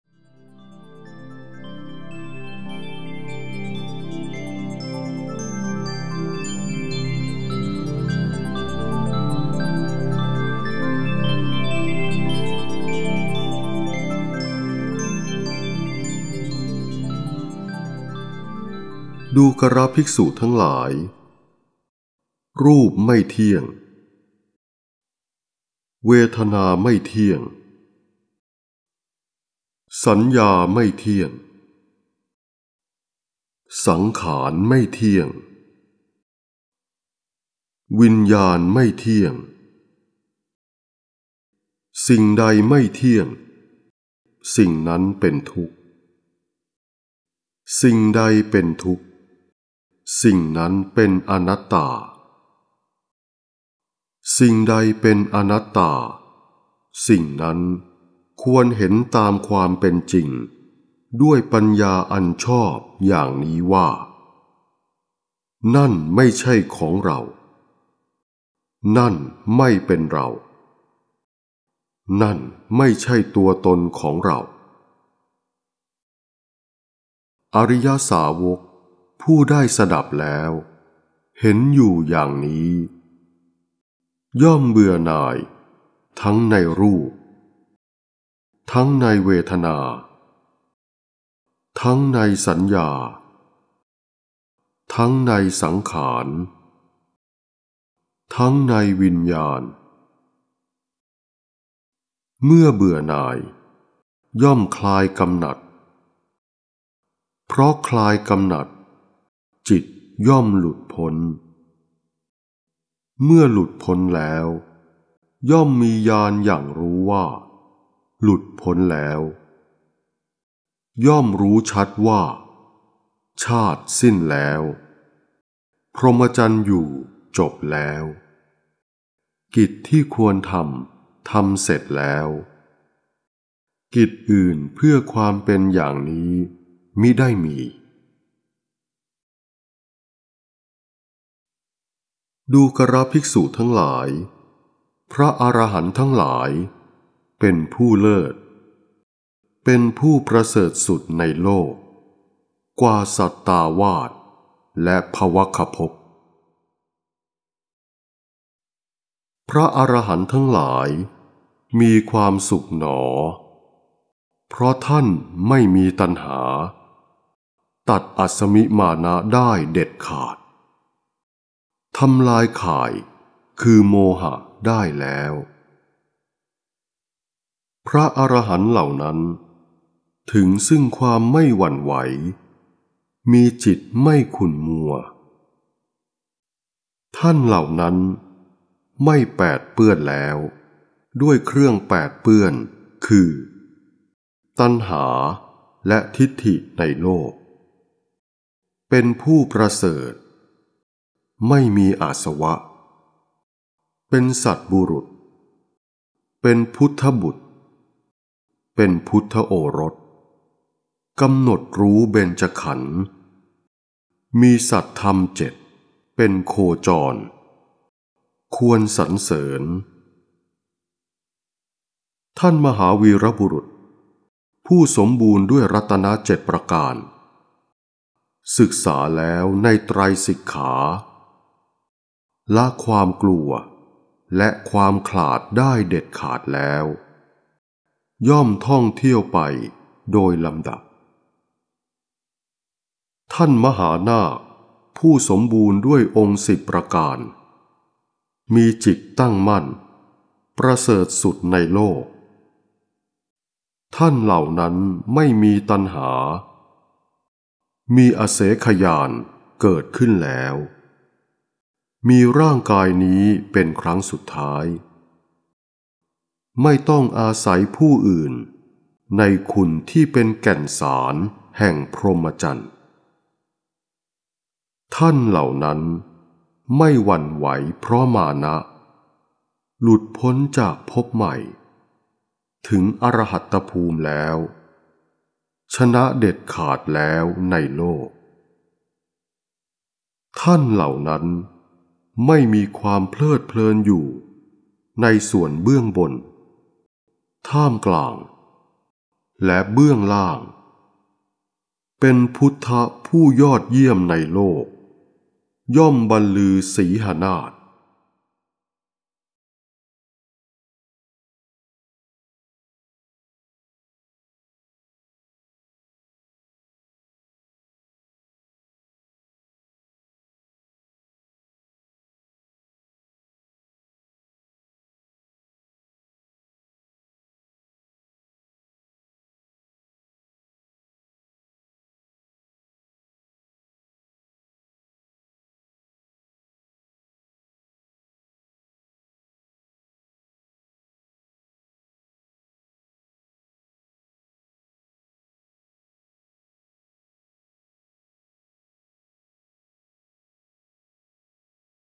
ปล่อยวางขันธ์ ๕ สู่อรหัตตผล | อรหันตสูตรที่ ๑
พุทธวิธีปฏิบัติธรรม
ปล่อยวางขันธ์ ๕ สู่อรหัตตผล
อรหันตสูตรที่ ๑
เล่มที่ ๑๗ ข้อที่ ๑๕๒-๑๕๓

ณ พระนครสาวัตถี พระผู้มีพระภาคตรัสกับภิกษุทั้งหลายว่า

ดูกรภิกษุทั้งหลาย
รูปไม่เที่ยง
เวทนาไม่เที่ยง
สัญญาไม่เที่ยง
สังขารไม่เที่ยง
วิญญาณไม่เที่ยง

สิ่งใดไม่เที่ยง สิ่งนั้นเป็นทุกข์
สิ่งใดเป็นทุกข์ สิ่งนั้นเป็นอนัตตา
สิ่งใดเป็นอนัตตา สิ่งนั้นควรเห็นตามความเป็นจริงด้วยปัญญาอันชอบ อย่างนี้ว่า นั่นไม่ใช่ของเรา นั่นไม่เป็นเรา นั่นไม่ใช่ตัวตนของเรา

อริยสาวกผู้ได้สดับแล้ว เห็นอยู่อย่างนี้ ย่อมเบื่อหน่ายทั้งในรูป ทั้งในเวทนาทั้งในสัญญา ทั้งในสังขาร ทั้งในวิญญาณ

เมื่อเบื่อหน่าย ย่อมคลายกำหนัด
เพราะคลายกำหนัด จิตย่อมหลุดพ้น
เมื่อหลุดพ้นแล้ว ย่อมมีญาณหยั่งรู้ว่า หลุดพ้นแล้ว
ย่อมรู้ชัดว่าชาติสิ้นแล้ว พรหมจรรย์อยู่จบแล้ว
กิจที่ควรทำ ทำเสร็จแล้ว
กิจอื่นเพื่อความเป็นอย่างนี้มิได้มี

ดูกรภิกษุทั้งหลาย พระอรหันต์ทั้งหลาย เป็นผู้เลิศ เป็นผู้ประเสริฐสุดในโลก กว่าสัตตาวาสและภวัคคภพ

พระอรหันต์ทั้งหลายมีความสุขหนอ
เพราะท่านไม่มีตัณหา
ตัดอัสมิมานะได้เด็ดขาด
ทำลายข่ายคือโมหะได้แล้ว
พระอรหันต์เหล่านั้นถึงซึ่งความไม่หวั่นไหว
มีจิตไม่ขุ่นมัว
ท่านเหล่านั้นไม่แปดเปื้อนแล้ว
ด้วยเครื่องแปดเปื้อนคือตัณหาและทิฏฐิในโลก
เป็นผู้ประเสริฐ ไม่มีอาสวะ
เป็นสัตบุรุษ เป็นพุทธบุตร เป็นพุทธโอรส
กำหนดรู้เบญจขันธ์มีสัทธรรม ๗ เป็นโคจร ควรสรรเสริญ.
ท่านมหาวีรบุรุษผู้สมบูรณ์ด้วยรัตนะ ๗ ประการ
ศึกษาแล้วในไตรสิกขา
ละความกลัวและความขลาดได้เด็ดขาดแล้ว ย่อมท่องเที่ยวไปโดยลำดับ

ท่านมหานาคผู้สมบูรณ์ด้วยองค์ ๑๐ ประการ
มีจิตตั้งมั่น ประเสริฐสุดในโลก
ท่านเหล่านั้นไม่มีตัณหา มีอเสขญาณเกิดขึ้นแล้ว
มีร่างกายนี้เป็นครั้งสุดท้าย
ไม่ต้องอาศัยผู้อื่นในคุณที่เป็นแก่นสารแห่งพรหมจรรย์

ท่านเหล่านั้นไม่หวั่นไหวเพราะมานะ
หลุดพ้นจากภพใหม่ ถึงอรหัตภูมิแล้ว
ชนะเด็ดขาดแล้วในโลก

ท่านเหล่านั้นไม่มีความเพลิดเพลินอยู่ในส่วนเบื้องบน
ท่ามกลาง และเบื้องล่าง
เป็นพุทธผู้ยอดเยี่ยมในโลก ย่อมบันลือสีหนาท

Line Official Account : @uttayarndham